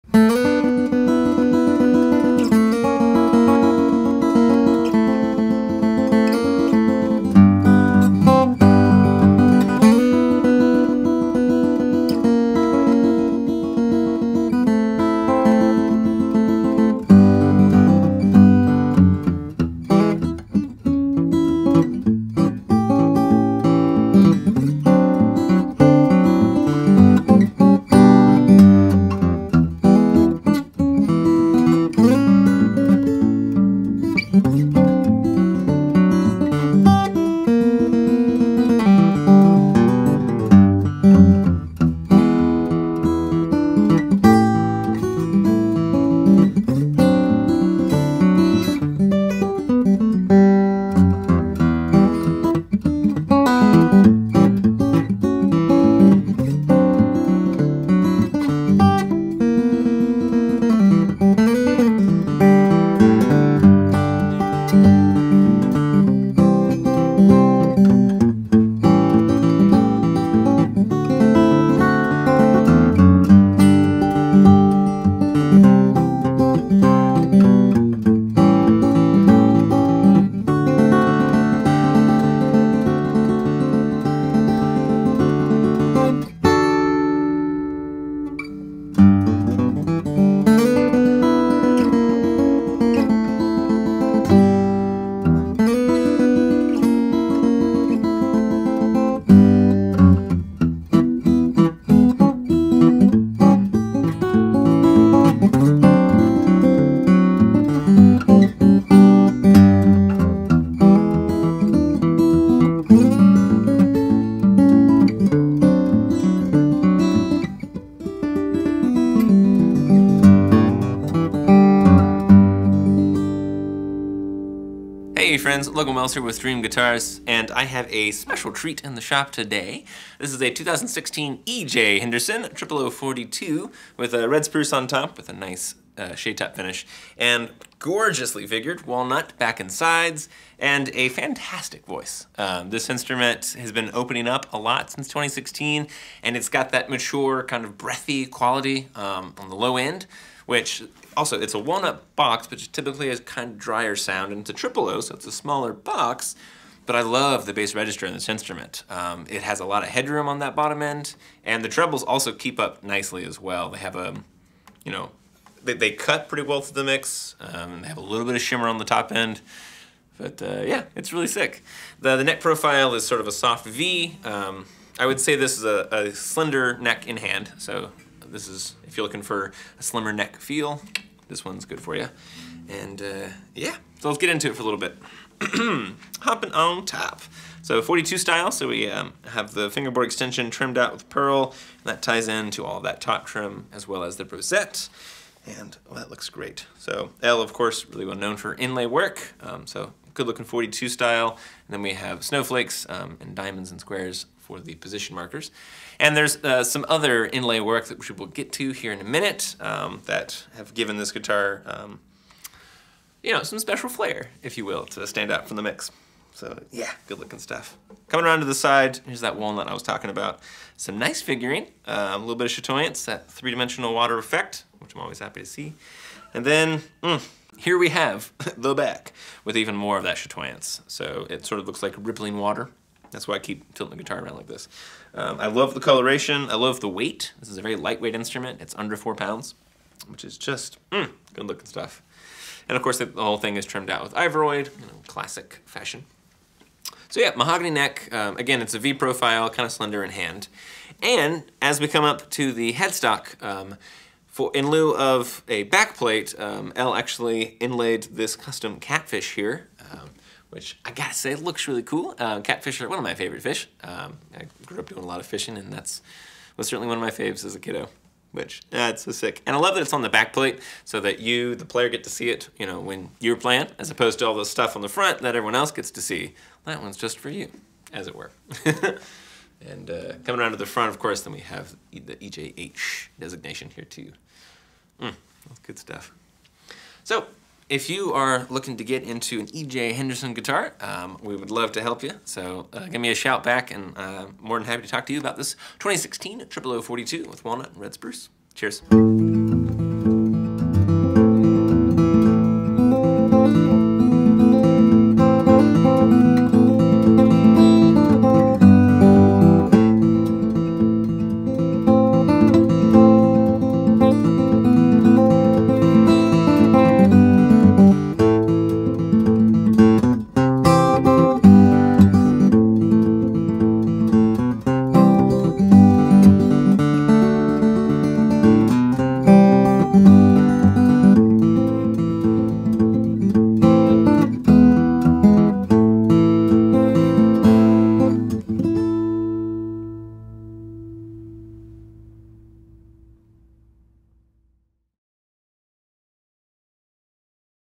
2016 EJ Henderson 000-42, Walnut & Red Spruce
Paul:Jayne has been building for years with her father, and is a skilled luthier in her own right these days. This 000 has great string energy and beautiful 42-style appointments.

Logan: Elle is a dear friend, and it's a treat to have one of her guitars come through the shop again. Walnut body and Red Spruce top, built in 2016 with 42-style appointments and a catfish at the headstock, this 000 has opened up for a quick and lively string energy and some sweet treble overtones with a fingerstyle approach.